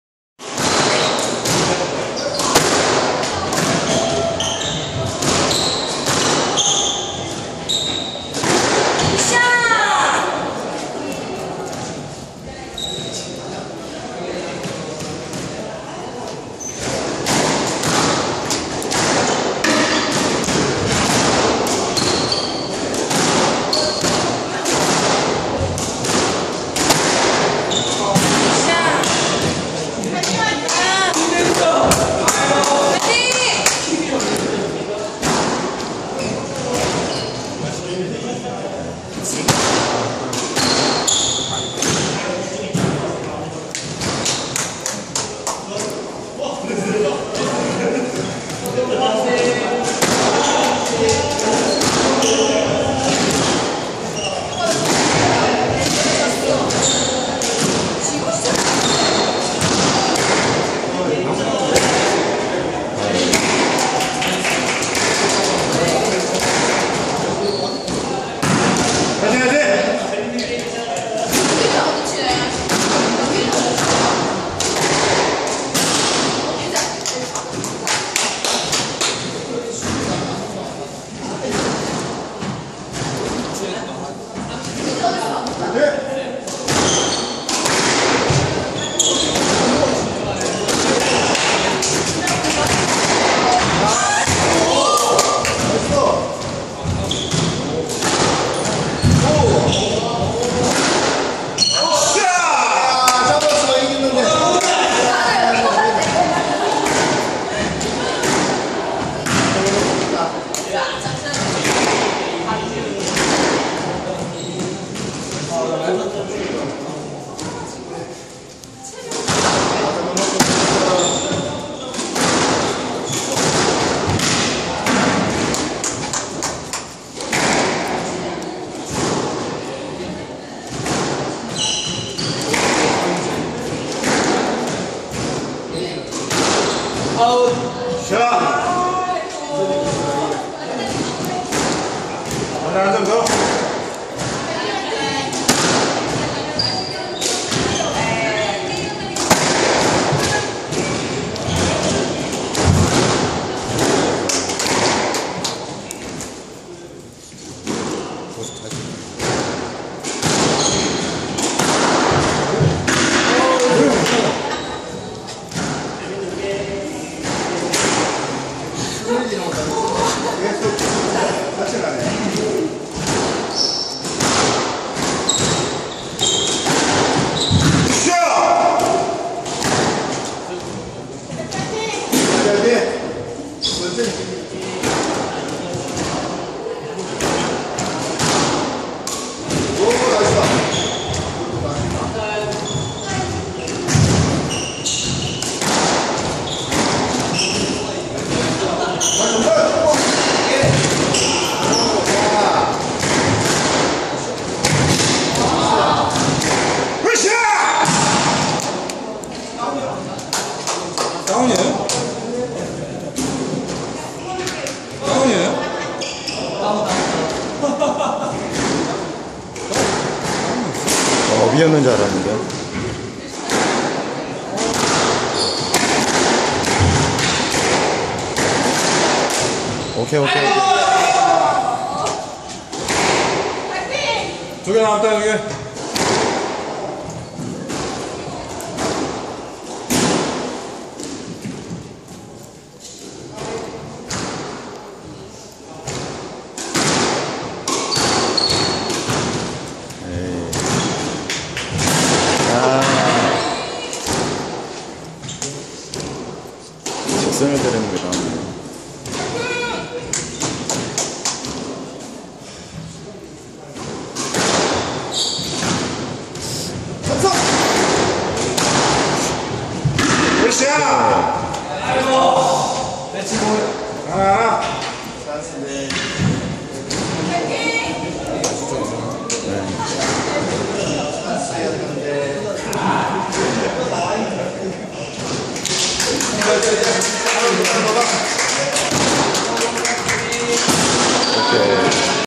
[HD] 스쿼시 제1회김천대회-김호성2회전3세트
본 동영상은 HD화질로 시청가능합니다.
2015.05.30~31
제1회김천생활스쿼시 전국동호인스쿼시대회
광주빛고을스쿼시 D.I.S(Do It Squash) 김호성
Korean Amateur Squash - Man / Woman / Mixed doubles